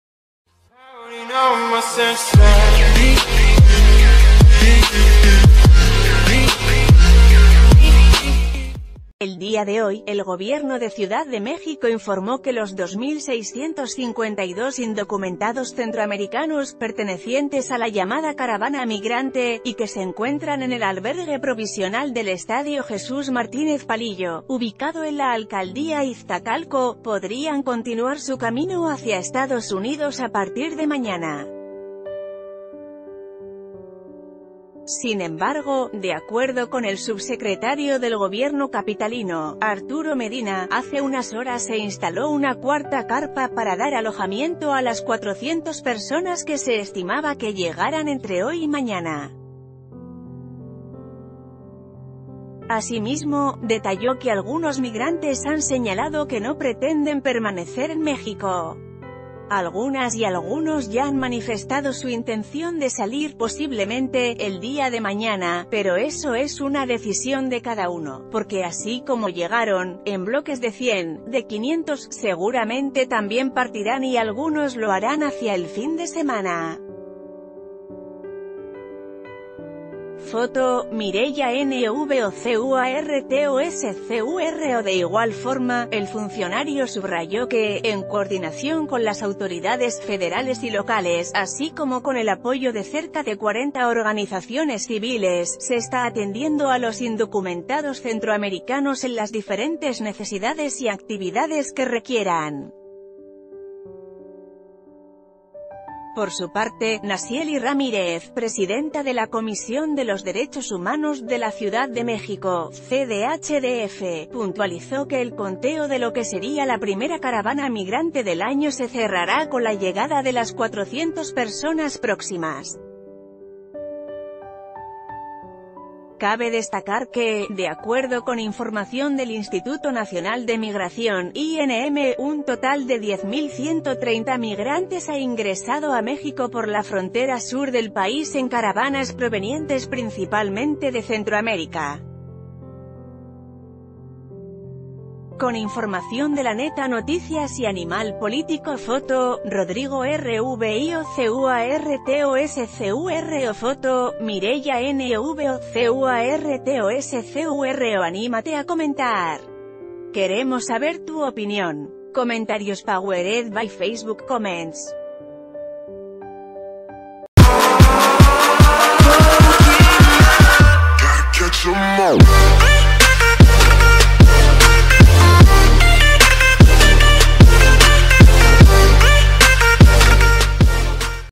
Algunos migrantes centroamericanos se van de CDMX, ¡pero vienen más en camino!
El día de hoy, el Gobierno de Ciudad de México informó que los 2 mil 652 indocumentados centroamericanos, pertenecientes a la llamada caravana migrante, y que se encuentran en el albergue provisional del estadio Jesús Martínez Palillo, ubicado en la alcaldía Iztacalco, podrían continuar su camino ha...